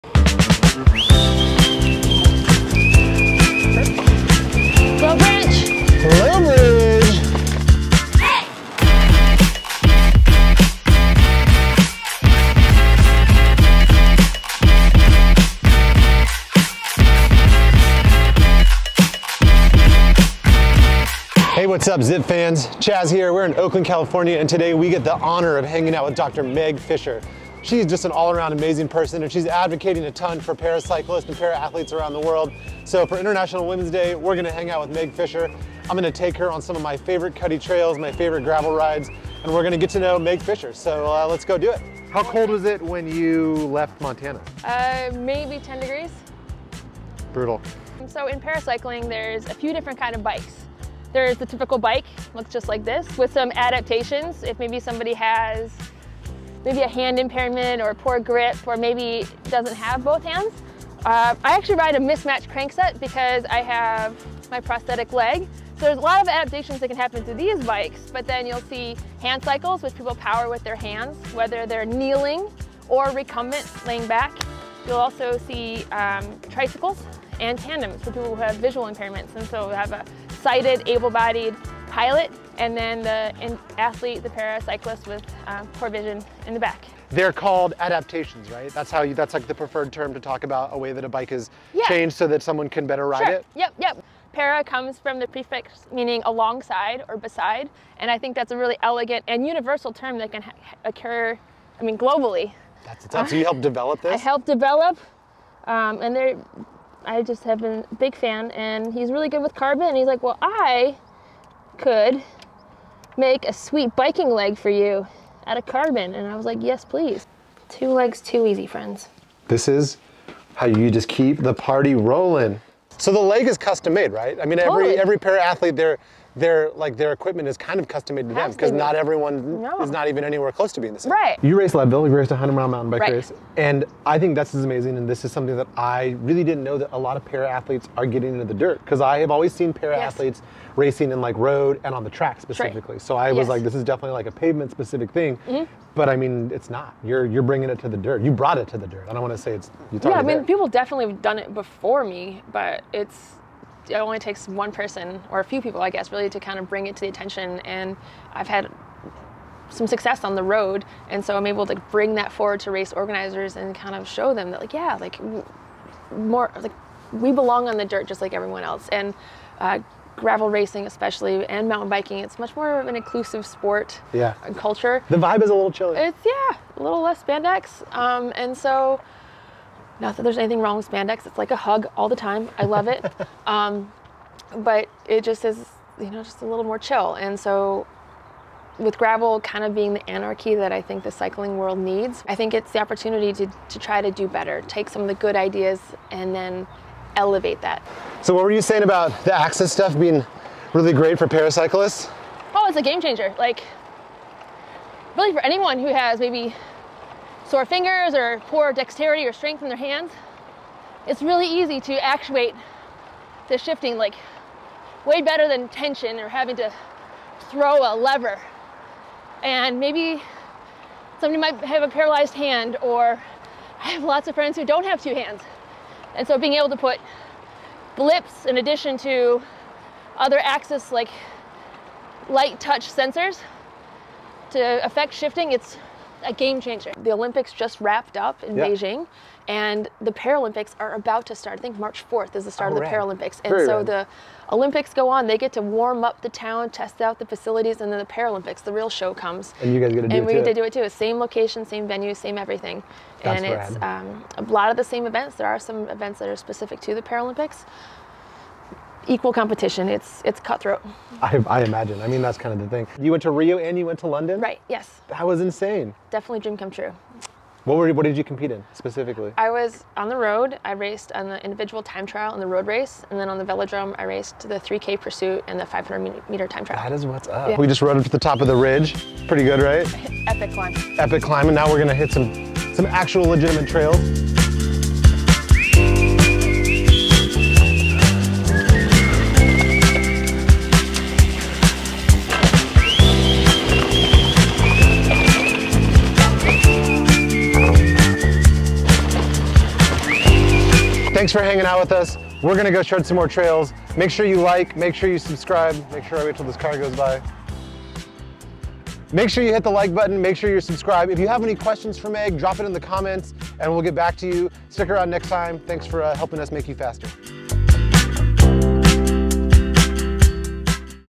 Meg Fisher: “Two Legs Too Easy!” | Zipp Making You Faster
In celebration of International Women’s Day, we’re hitting the dirt with Paralympic Gold Medalist, Dr. Meg Fisher. Meg is a Doctor of Physical Therapy, Athletic Performance Coach, and Motivational Speaker who is currently working to help expand Paracycling categories at some of the nation’s biggest off-road events. Follow along with Chas and Meg to learn more about the Olympian/World Champion/all-around inspiring human.

Follow the link below to learn more about Meg’s story